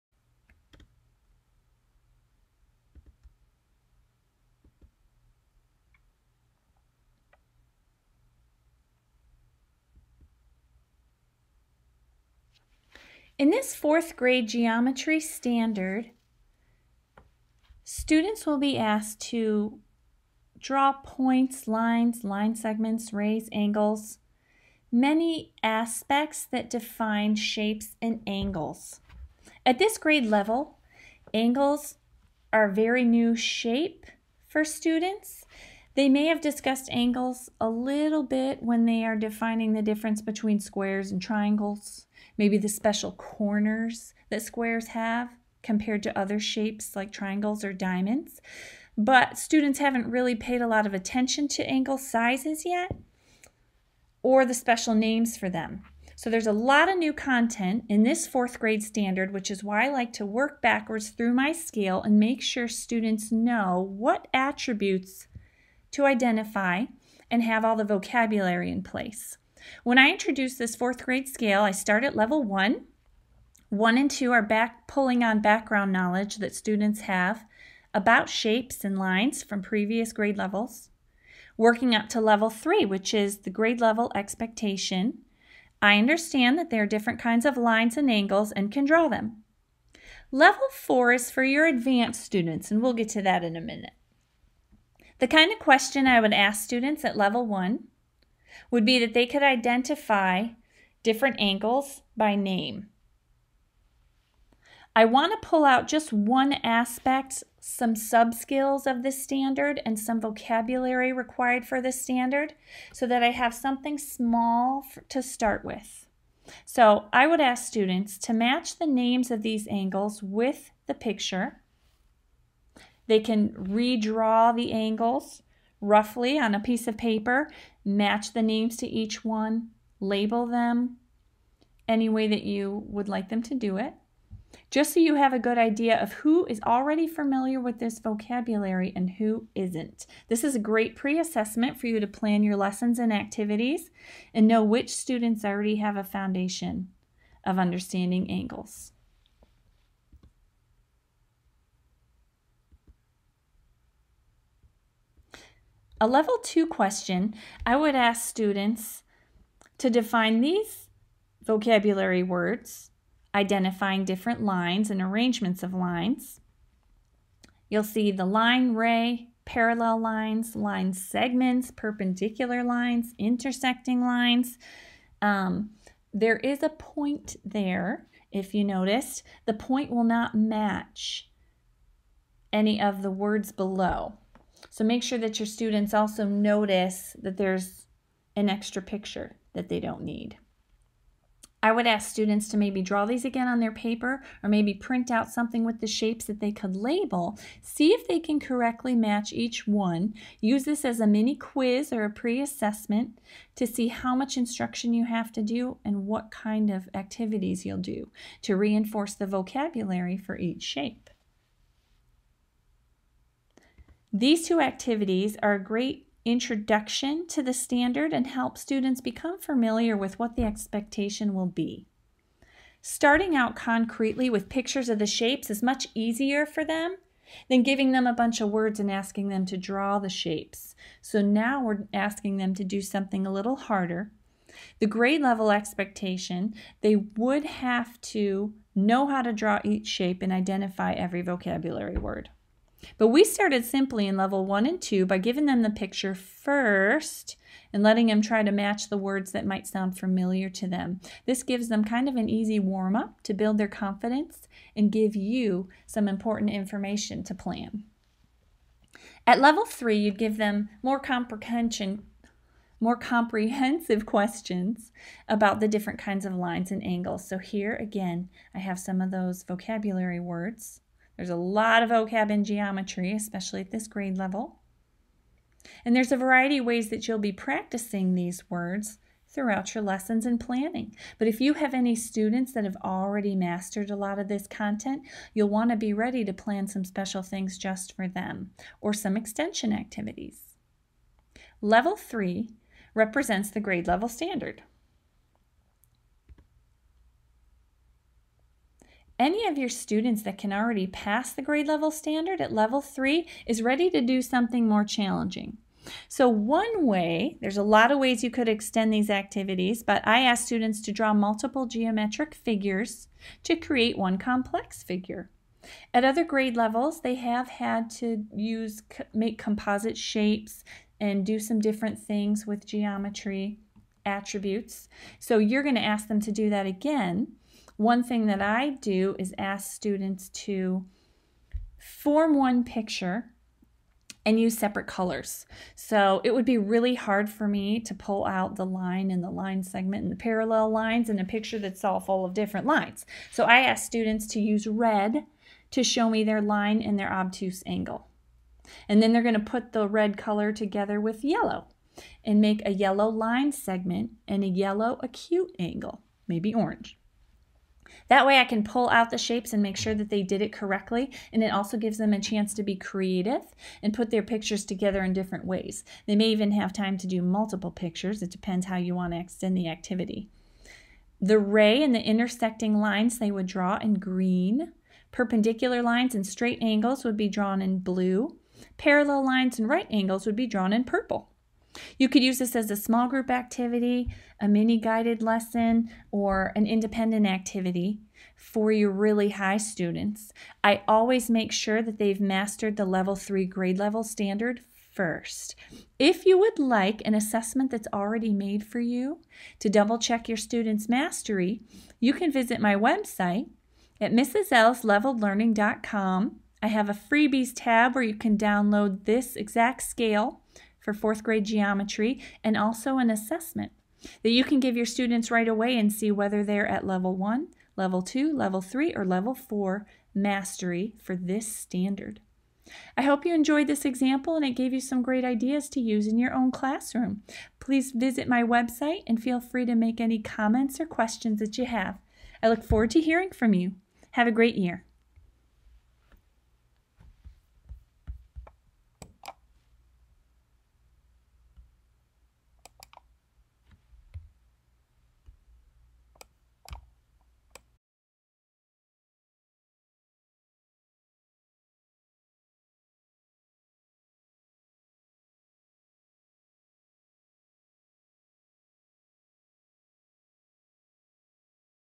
How to Teach Fourth Grade Geometry to Different Ability Levels
4G1 Geometry Differentiation for Fourth Grade Math - This video walks you through one way to pre-assess your math students in this Geometry standard. The standard is embedded in a scale with four related levels of difficulty. Level 1 being the easiest, level 2 being just below grade level, level 3 at grade level, and level 4 above grade level. These scales and assessment tasks were designed by Melanie LiCausi.  To LEARN MORE about Differentiation with Proficiency Scales, visit... www. mrslsleveledlearning.com